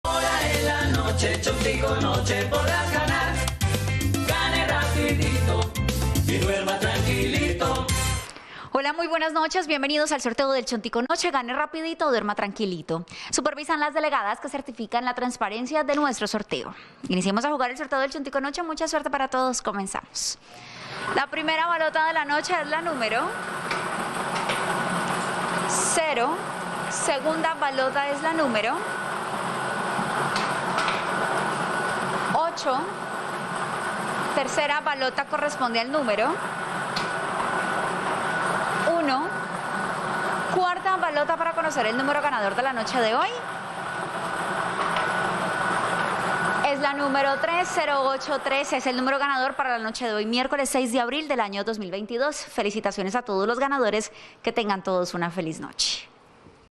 CHONTICO NOCHE Resultado del MIÉRCOLES 06 de abril de 2022 ✅🥇🔥💰
Hola buenas noches, te presentamos los resultados de las loterías y chances en Colombia, vamos con el Resultados del Chance El Chontico noche del MIÉRCOLES 06 de abril de 2022.

Resultado Lotería del Meta MIÉRCOLES 06 de abril de 20222

Resultados Lotería del Valle, Lotería del Meta, Lotería de Manizales y sorteo Astro Luna